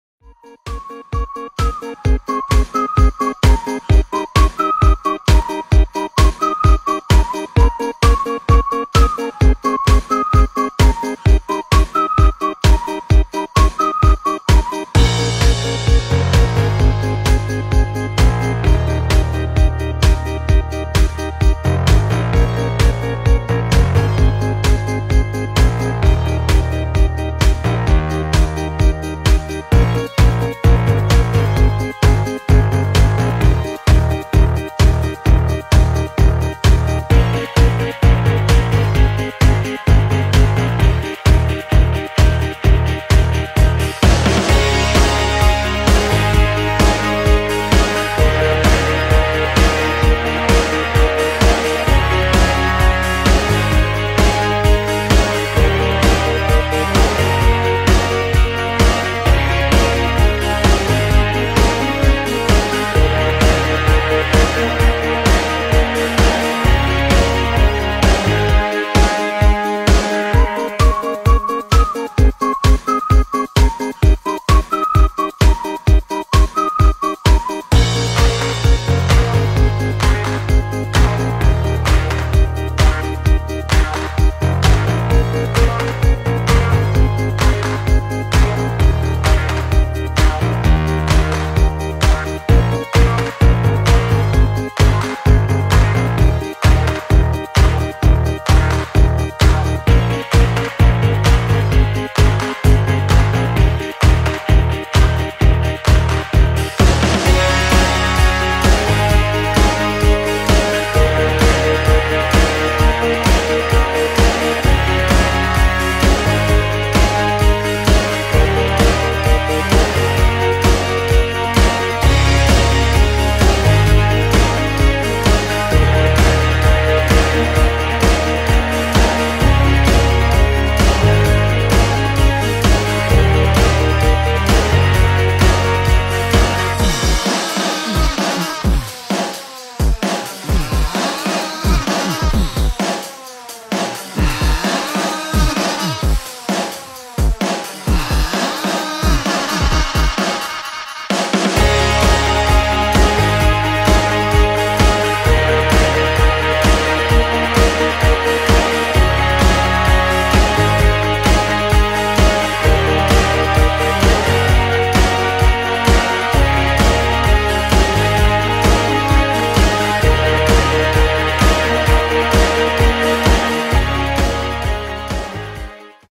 Woman Paints Image of Dog Chasing Duck - 986503-3
This artist used oil paint to create an image of a dog chasing a duck. After sketching out her image, she slowly added small dabs of paint to create her final piece.